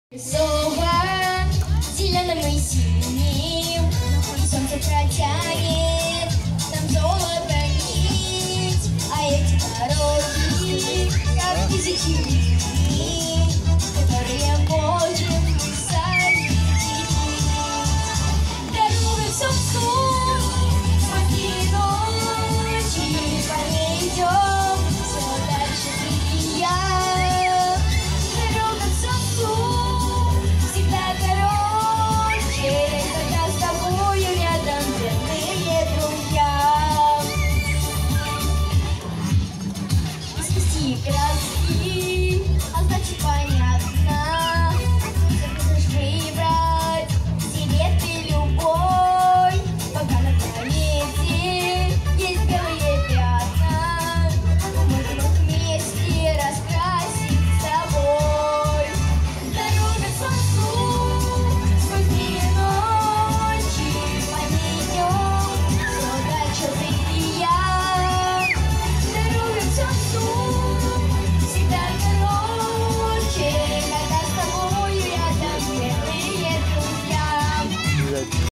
МЖК-1 5
Сейчас стало популярным объединяться с соседями, устраивать праздники двора, а в Свердловском МЖК-1 (сорокалетие которого отметили два года назад) такие вещи происходили вовсю ещё в прошлом веке. И вот сейчас здесь пытаются их вновь возродить. Тем более, что отличное место для проведения - каменный МЖКовский форум - был отреставрирован к юбилею.
В минувший понедельник там отметили окончание лета сборным концертом "и млада, и стара".
Екатеринбург.
29.8.22.